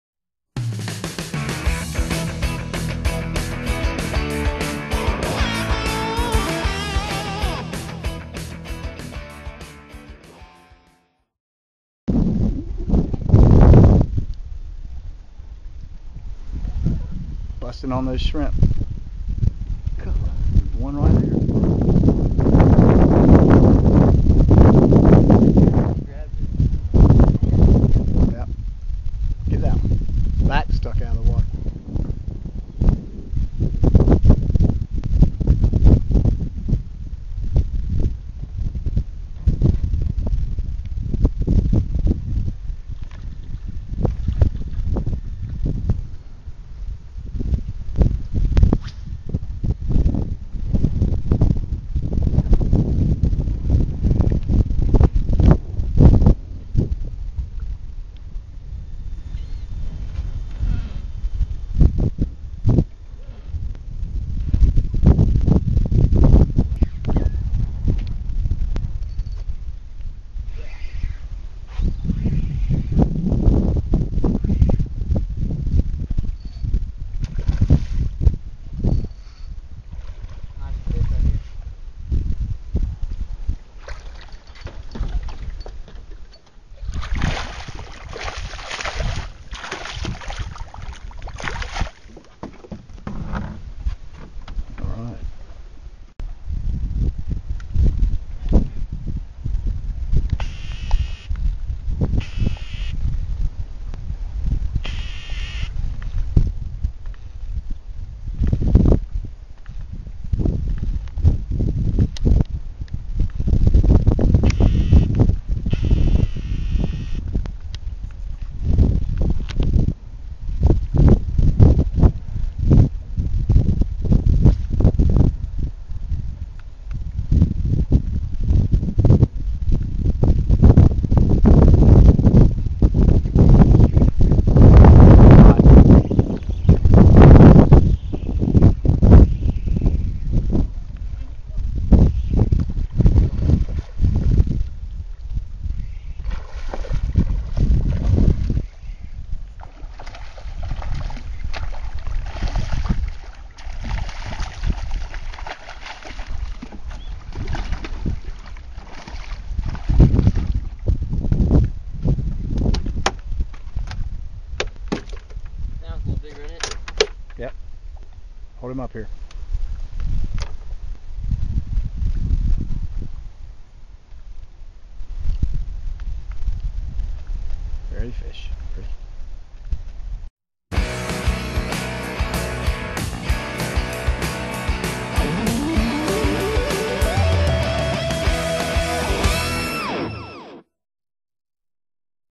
Creek reds
Creek reds with Capt. Jeff Wolfe of Seahawk Inshore Fishing Charters.